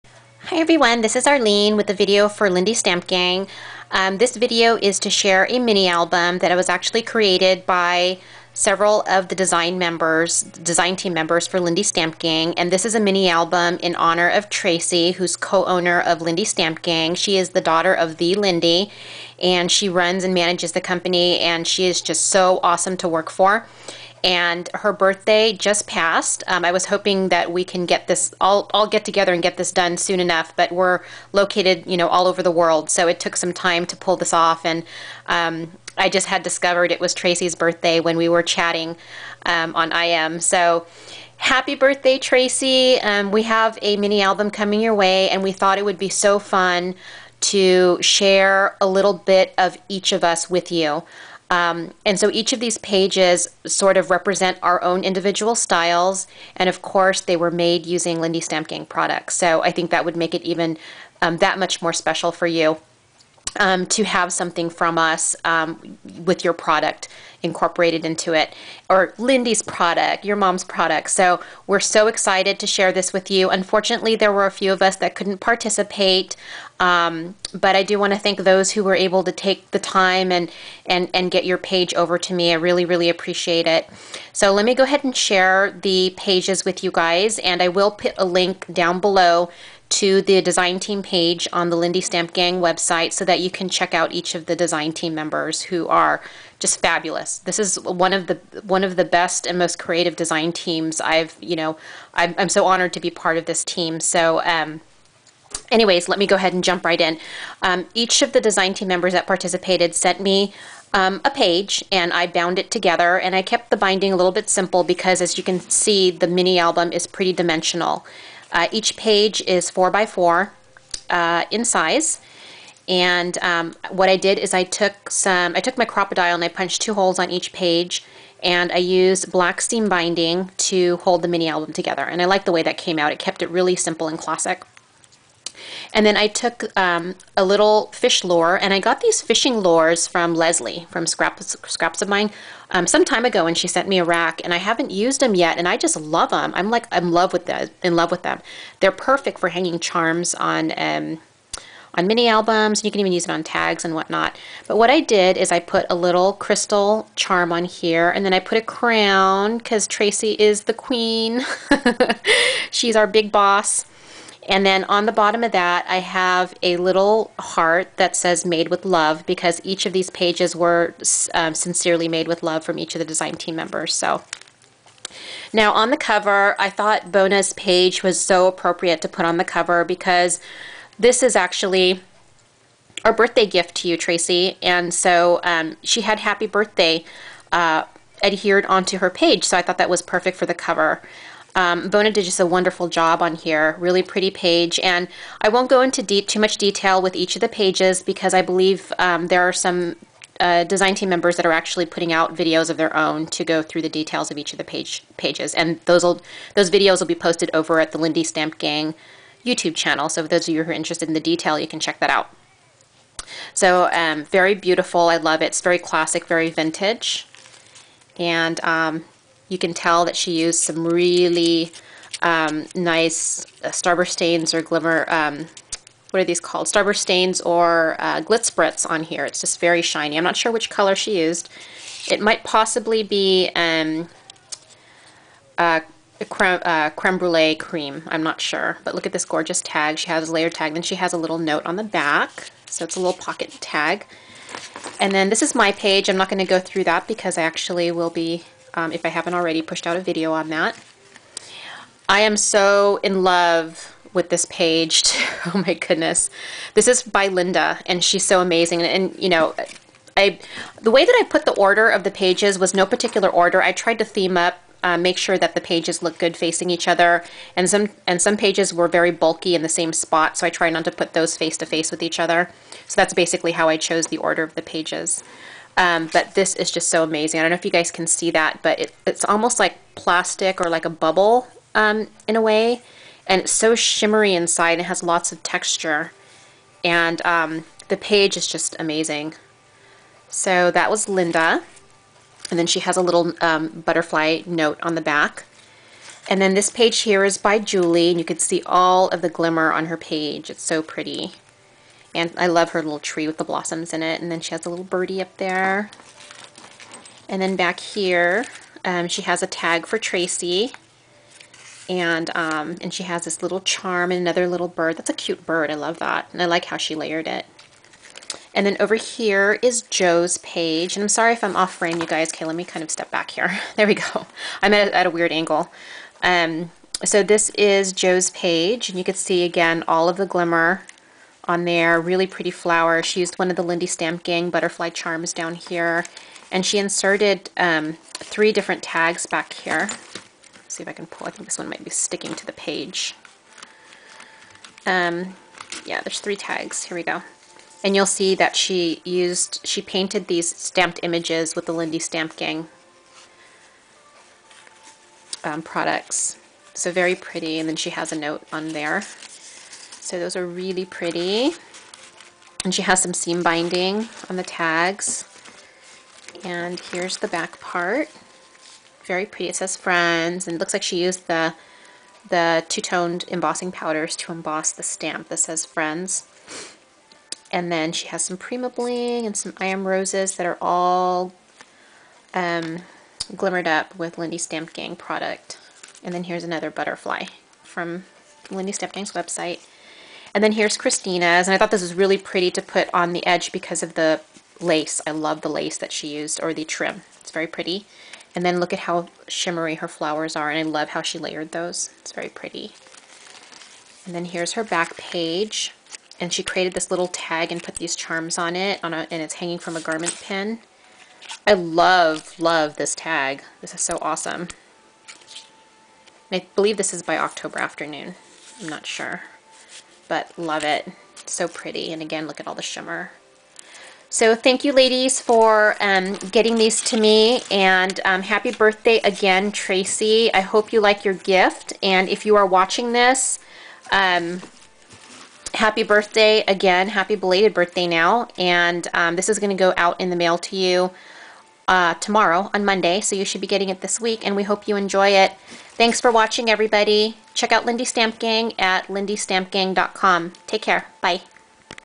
Birthday Mini Album for Tracey of Lindy Stamp Gang
The Lindy Stamp Gang design team got together to create this mini album in honor of Tracey's birthday. Tracey is the daughter of "the" Lindy of Lindy Stamp Gang and she is the most amazing boss to work for! Each page of this mini album is created by the unique LSG design team members.

We hope you like your mini album Tracey -- Happy Birthday!!

Design Team Members who contributed pages:

Bona, Arlene, Linda, Julie, Jo and Christina.